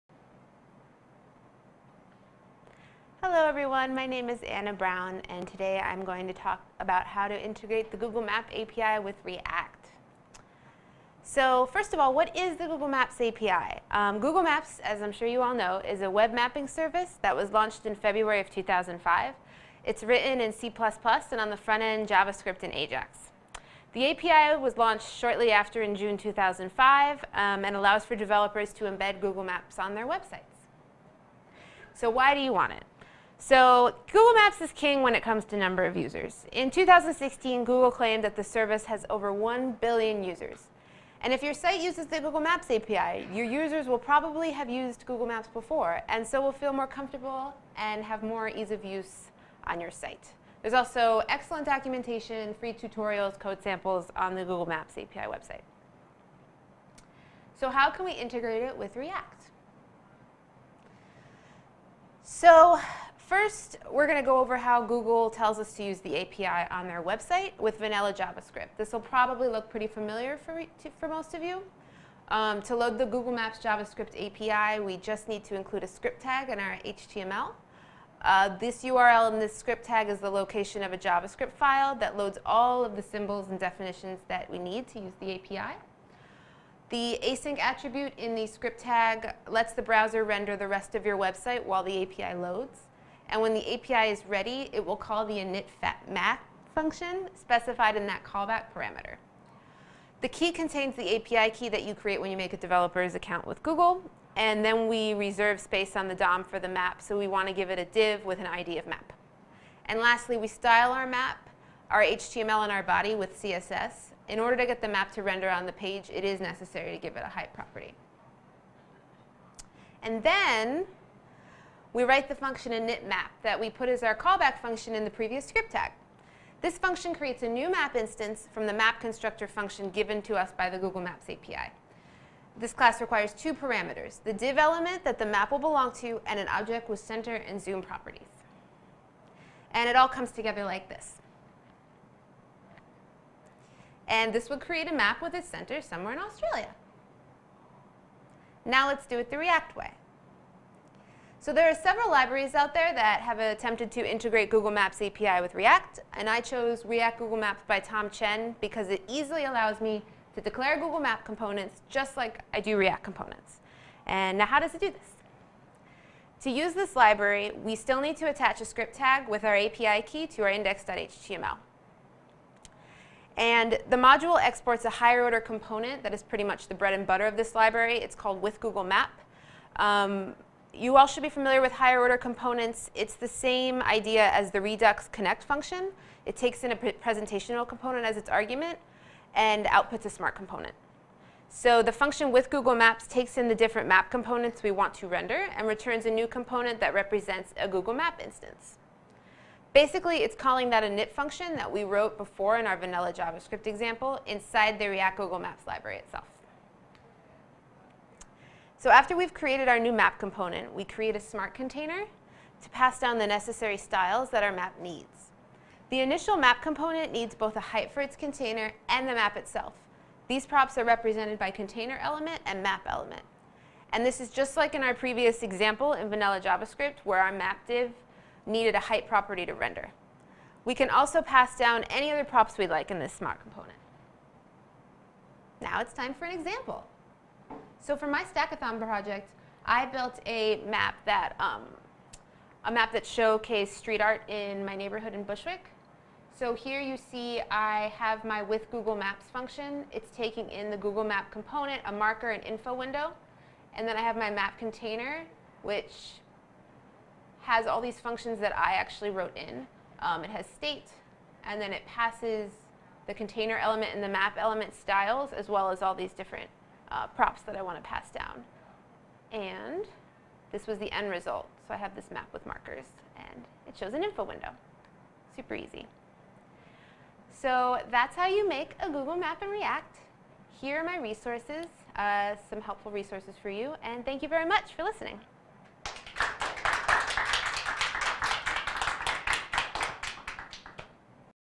How to Make a Google Map with React.js? - A Step-by-Step Tutorial
The Google Maps API allows developers to embed Google Maps onto their websites. In this tutorial, we walk you through how to integrate maps into a React application using the npm library react-google-maps, to gain access to the Google Maps API and all the robust functionality it provides.

Watch this video to learn:

- What is the Google Maps API
- Why you would want to use it
- How to make a Google Map with React